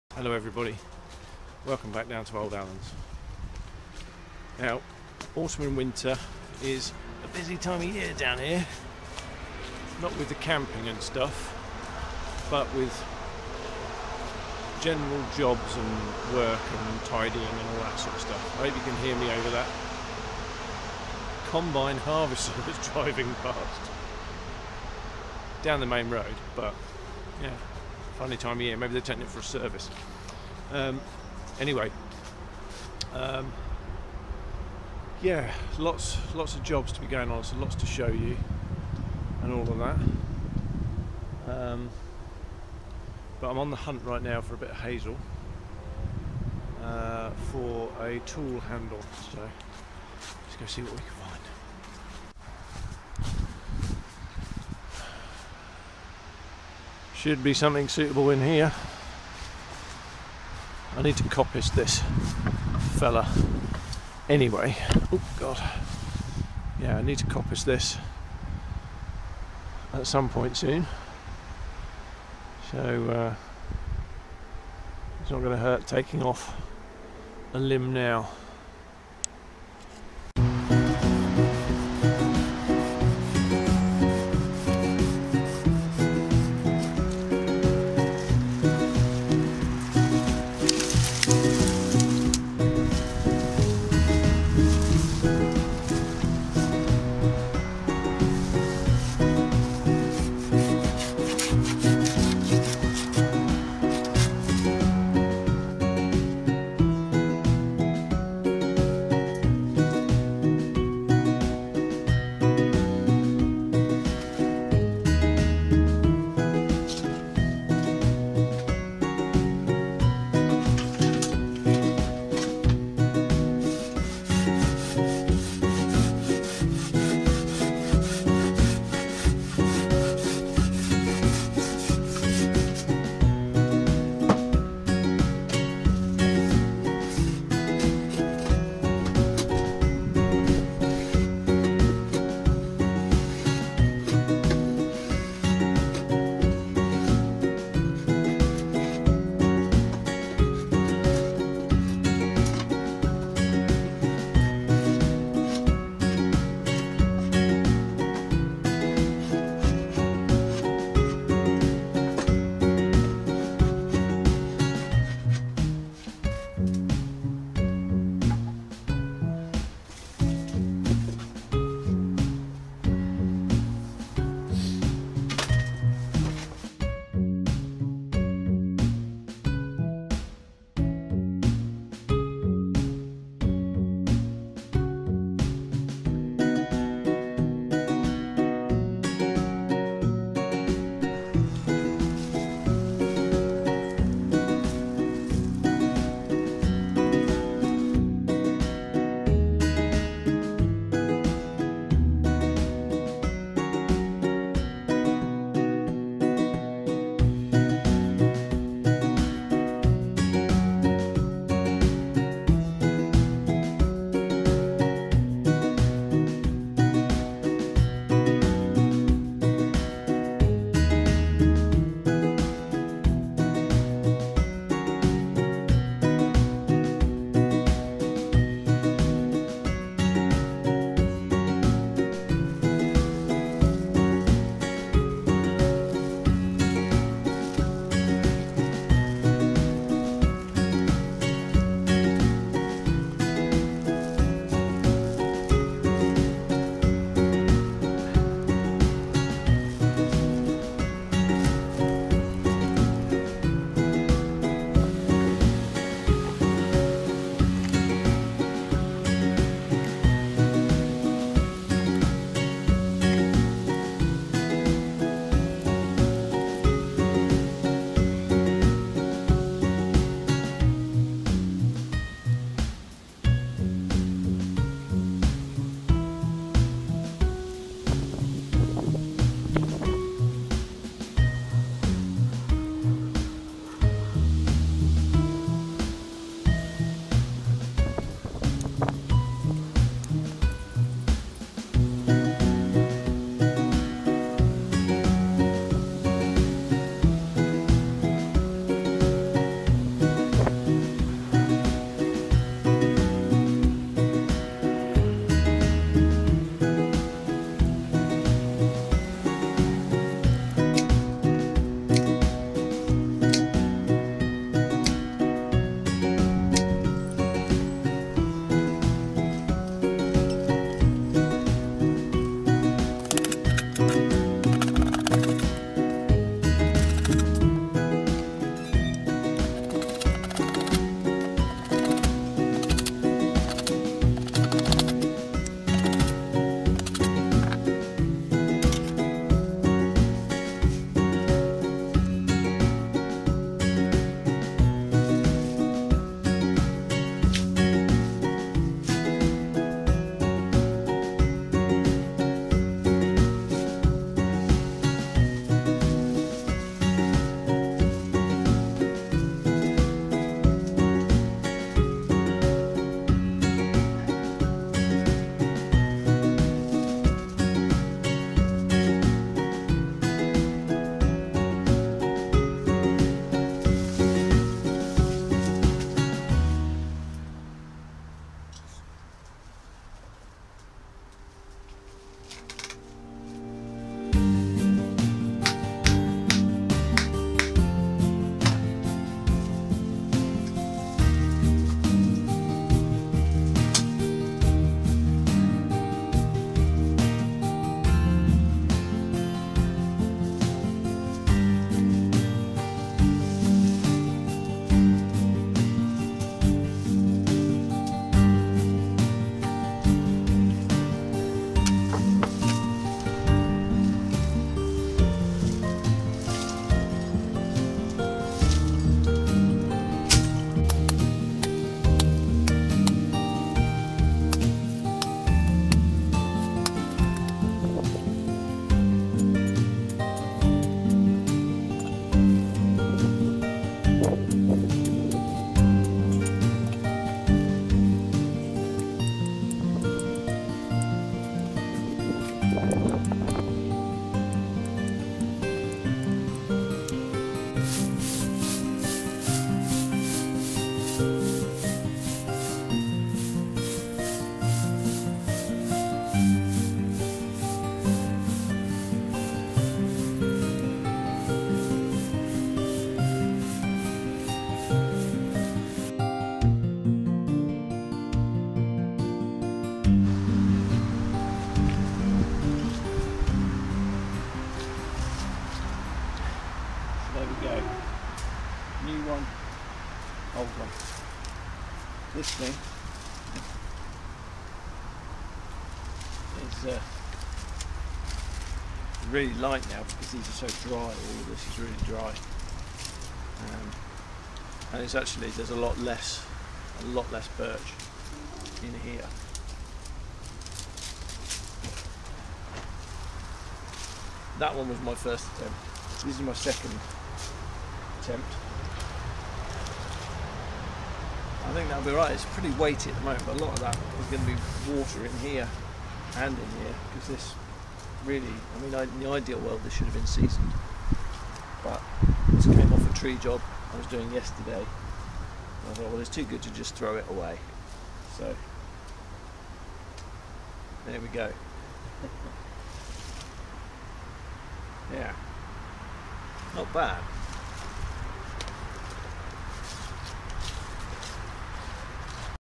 Making a Besom Broom
Pruning a Birch tree I planted 20 years ago meant I had some spare twings to make a new broom, I'm Certainly no expert but I do like playing in the woods. I hope you enjoy.